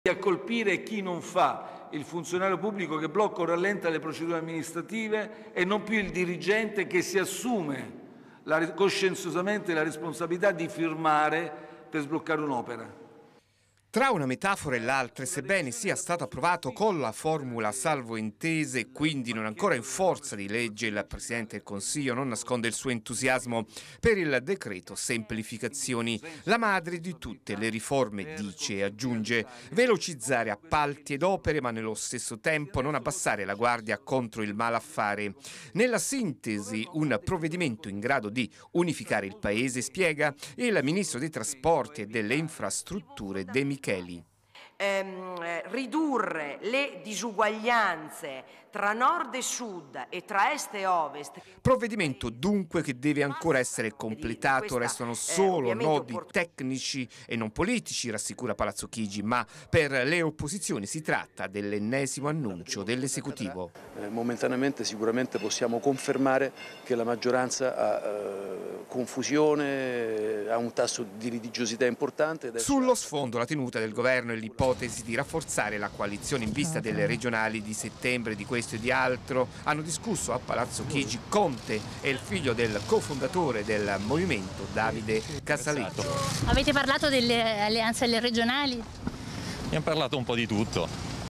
Governo, Conte lancia decreto semplificazioni per uscire dalla crisi post Covid-19
Per uscire dalla crisi innescata dalla pandemia la ricetta del governo è il decreto semplificazioni presenta oggi alla stampa da Conte. Servizio di Augusto Cantelmi